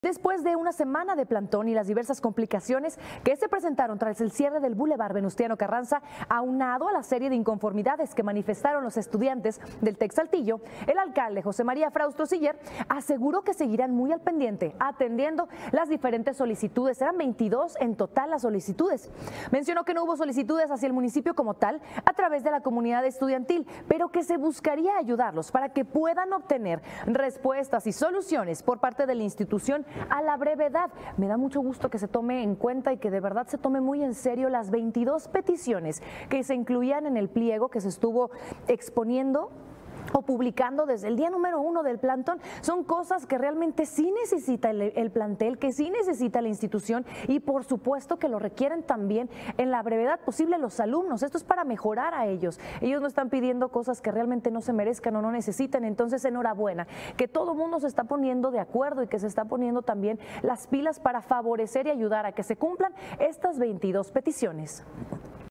Municipio apoyará a estudiantes del Tecnológico de Saltillo
Local | Después de una semana de plantón por parte de los alumnos del Instituto Tecnológico de Saltillo, el alcalde José María Frausto Siller aseguró que seguirán al pendiente atendiendo las diferentes solicitudes.